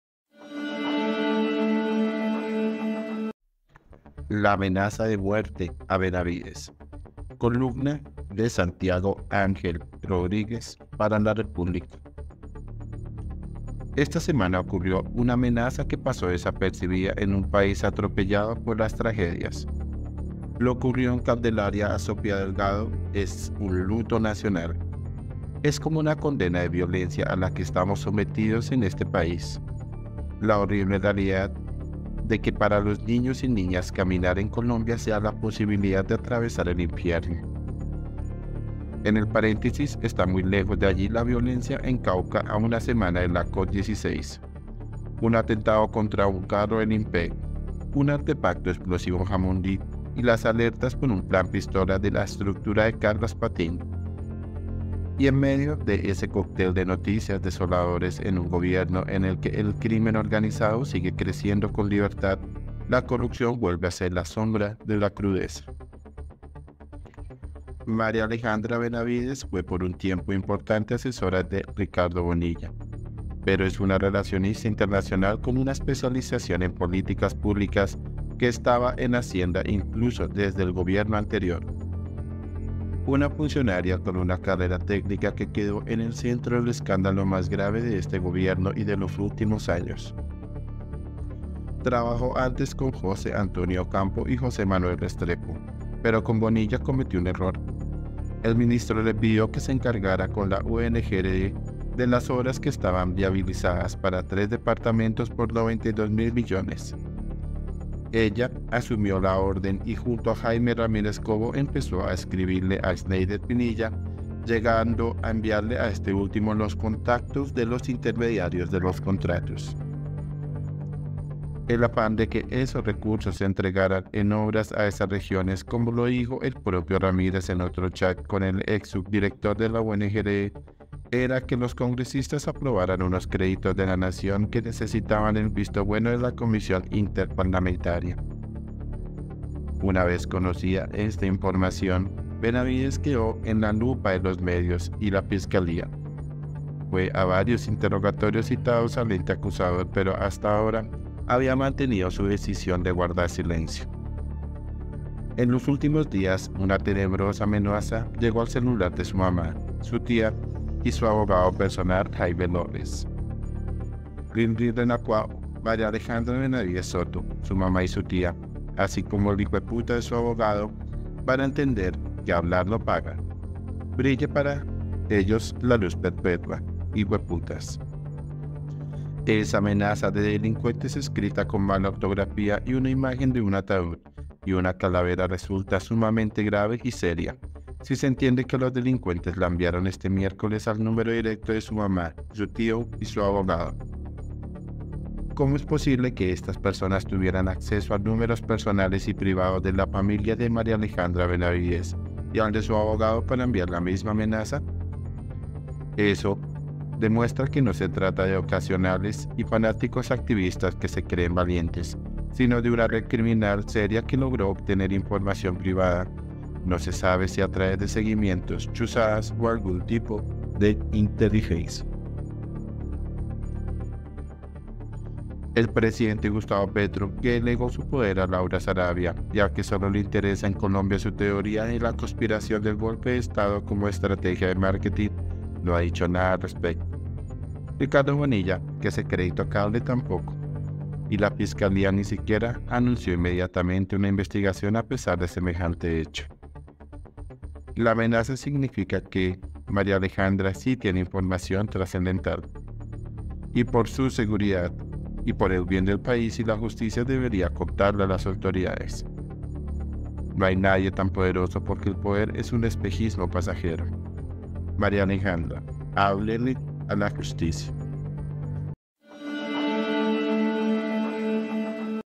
MARÍA ALEJANDRA HÁBLELE A LA JUSTICIA ⛔ GRAVE DENUNCIA || Columna Santiago Ángel Rodríguez
El presidente Gustavo Petro, que delegó su poder a Laura Sarabia, y al que solo le interesa en Colombia su teoría de la conspiración del golpe de Estado como estrategia de marketing, no ha dicho nada al respecto. Ricardo Bonilla, que se cree intocable, tampoco. Y la Fiscalía ni siquiera anunció inmediatamente una investigación a pesar de semejante hecho.
columna SANTIAGO ÁNGEL RODRÍGUEZ para LA REPÚBLICA